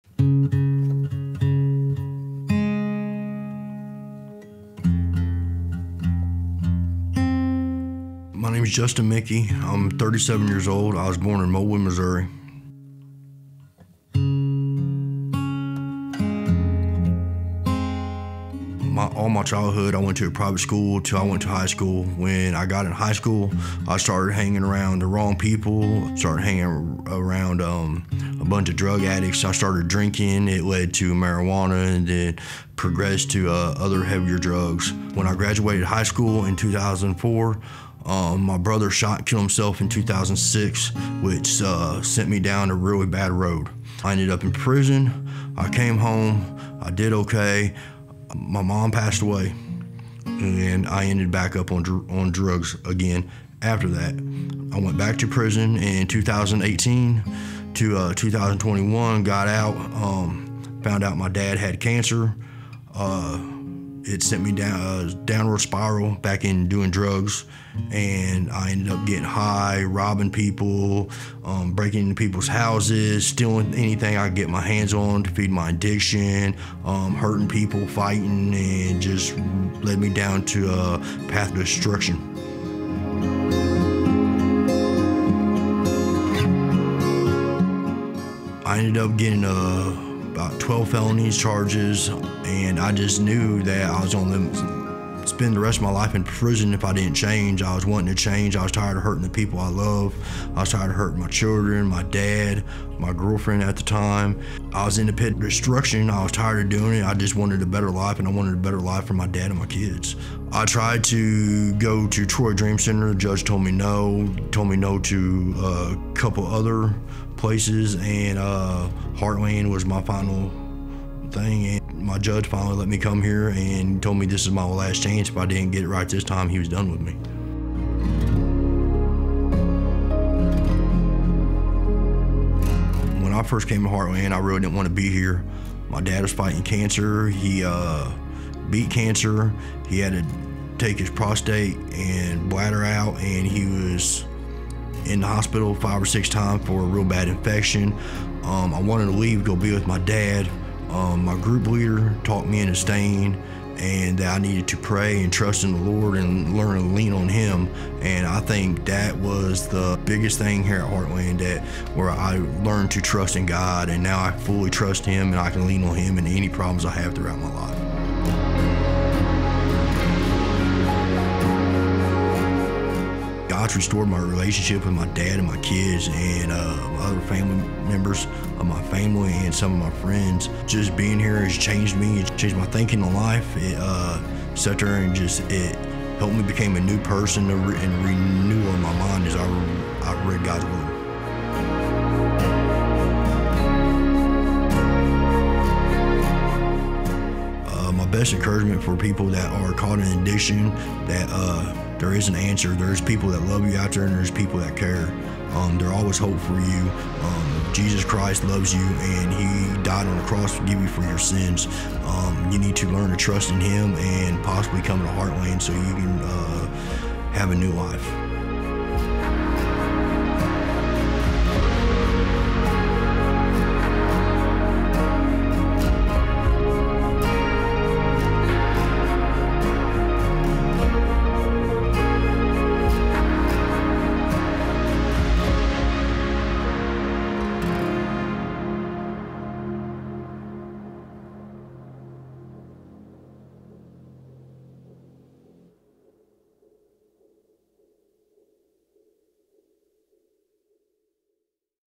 Walking Through Recovery | Justin's Story
Justin grew up going to a private school. However, his high school experience took a turn for the worse as he "started hanging around the wrong people... progressed to other heavier drugs."

A few years after graduating high school Justin lost his brother to suicide. This loss started Justin down "a really bad road" marked by imprisonment and relapses. Even intermittent attempts at rehabilitation were thwarted by his mother's demise and his father's battle with cancer, prompting a return to addiction and criminal behavior, resorting to theft and violence to support his addiction. As the felony charges started to pile up, Justin realized that he was going to spend " the rest of my life in prison if I didn't change."

A judge gave Justin one last opportunity for redemption through the Heartland program. Justin began to "trust in the Lord and... lean on him." instead of turning to drugs to deal with life. God has begun to restore relationships as Justin has leaned on Him. Justin shared that, "there is an answer... Jesus Christ loves you and he died on the cross to give you from your sins."

Contact Heartland Recovery and take the first step towards healing:
Heartland Men’s Recovery Program
500 New Creation Road Newark, MO 63458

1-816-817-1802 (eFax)

MB0154PYY51KANR